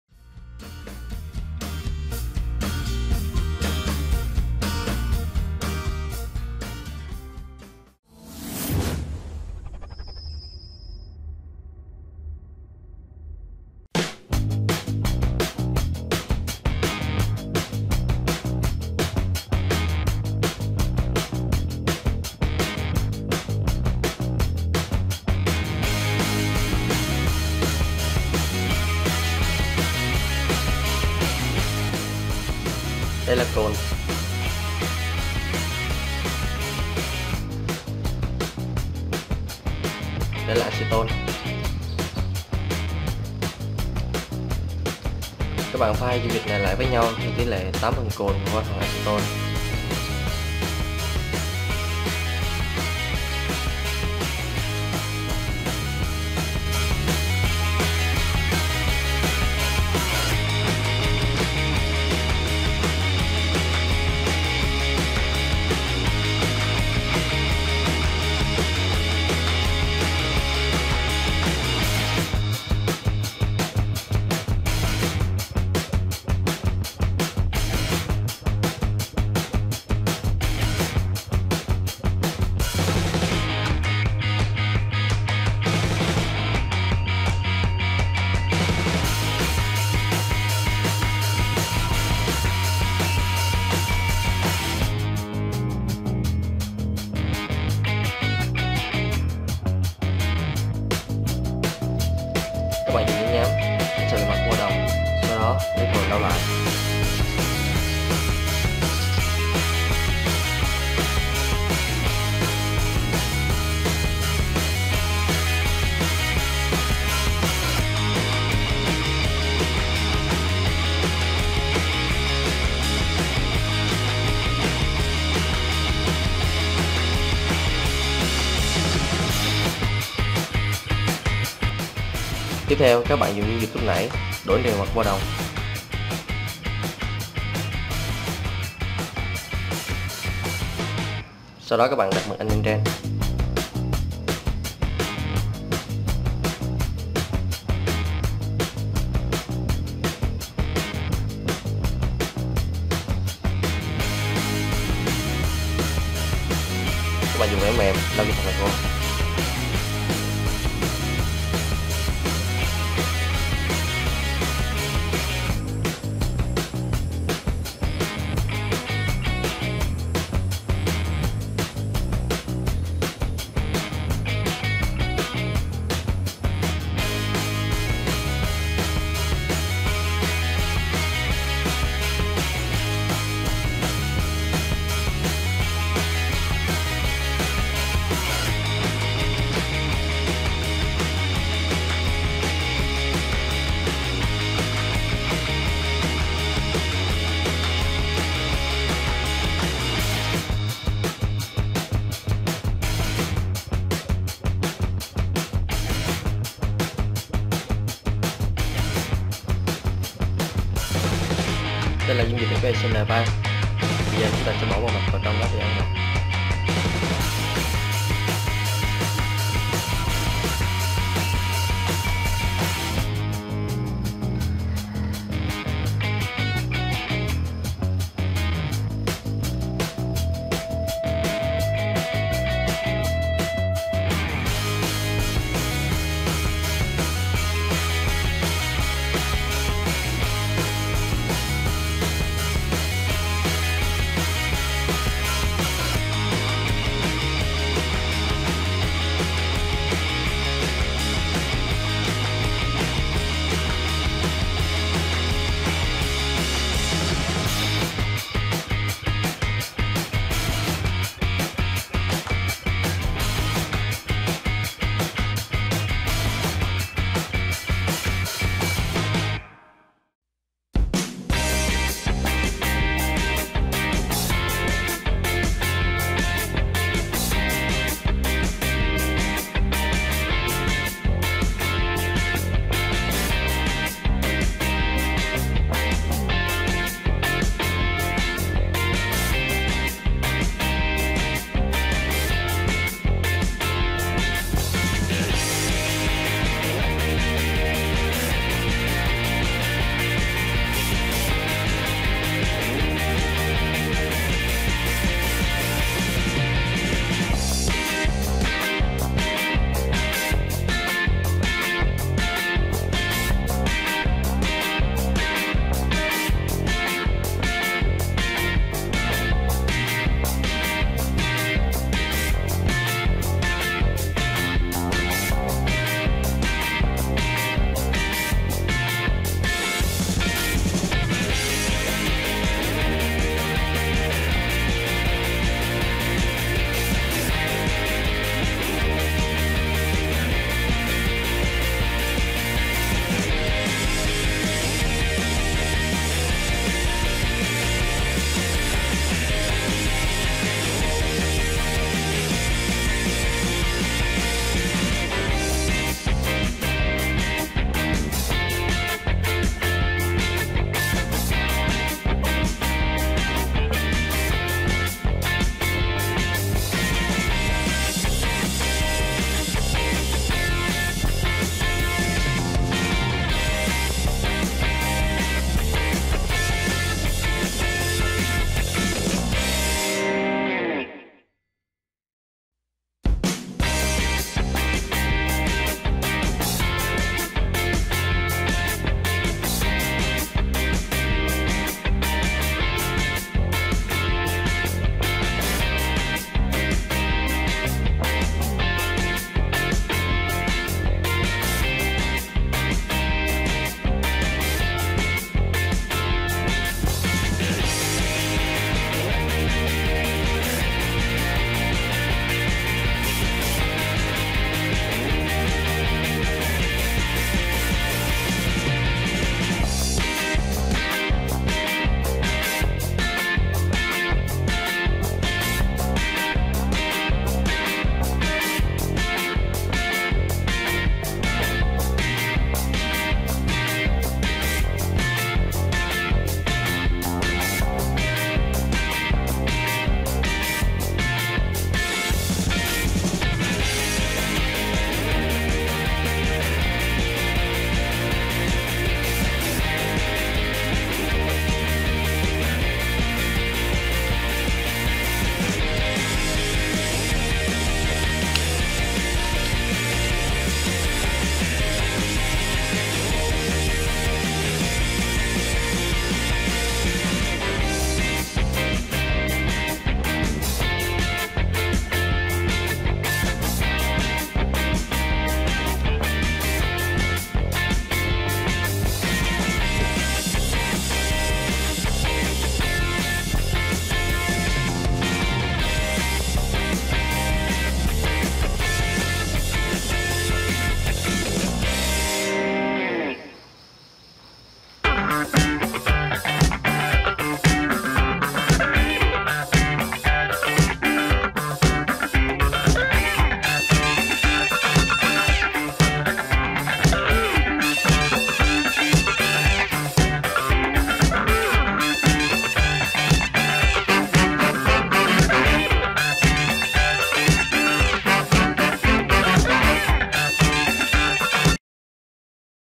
Hướng Dẫn Làm Mạch In Không Cần Bàn Ủi ! Cực Kì Đơn Giản ! Demo Mạch Nạp Pin Lion 18650
Chào các bạn đã đến Kênh Sáng Tạo  PL
Mình sẽ hướng dẫn cho các bạn một phương pháp làm mạch in mới,cực kỳ đơn giản, không cần dùng bàn ủi. Demo mạch nạp pin lion 18650.

Chuẩn bị:
- Dung dịch acetone (các bạn mua ở các cửa hàng,loại này rất thông dụng)
- Fecl3 (các bạn mua  ở cửa hàng điện tử hoặc hóa chất)
- Cồn 90 độ (các bạn mua ở các quầy thuốc.
- Board đồng
- Giấy nhám.
- IC MT 3582DA
- 2 tụ 104
- Led xanh
- Led đỏ
Bước 1: Tạo dung dịch chuyển mạch
Bước 2: chuyển mạch in lên  board đồng
Bước 3: Ăn mòn
Bước 4: Khoan và hàn linh kiên
Bước 5: Thử mạch nạp pin lion